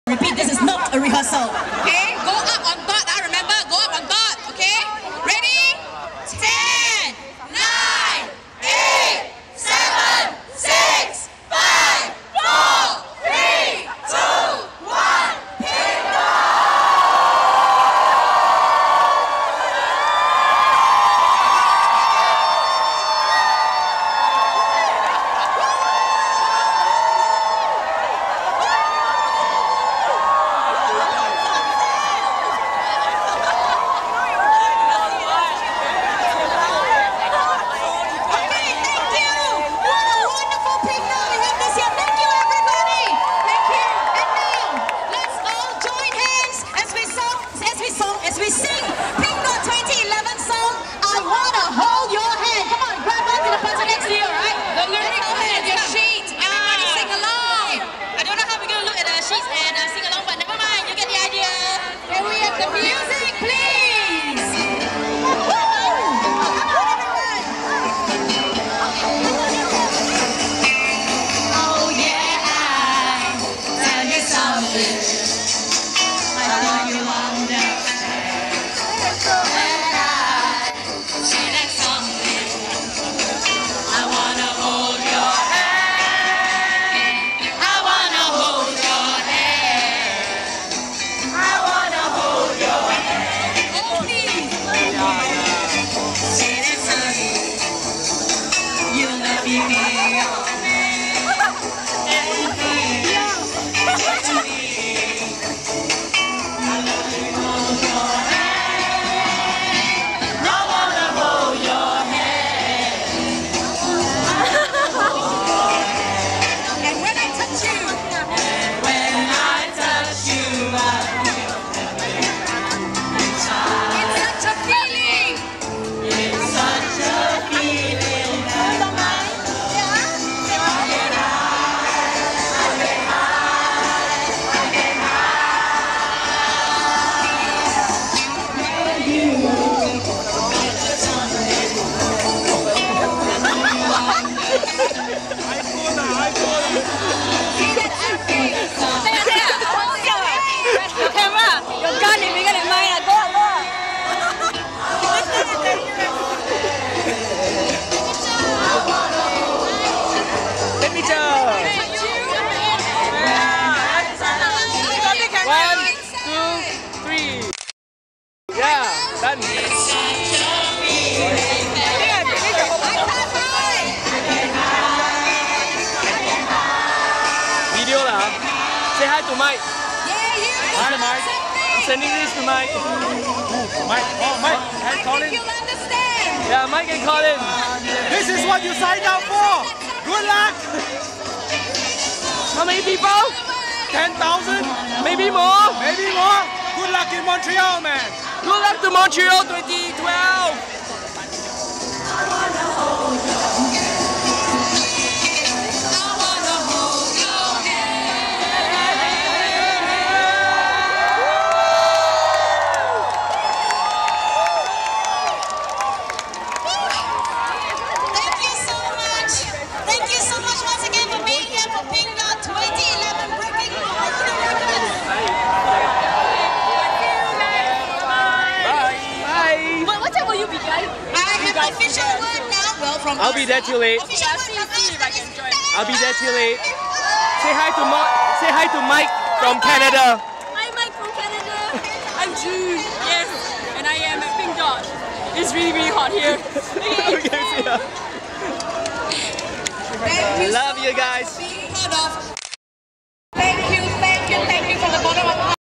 Pinkdot 2011 (As seen by me!)
Over 10,000 people gathered at Hong Lim Park to support our freedom to love.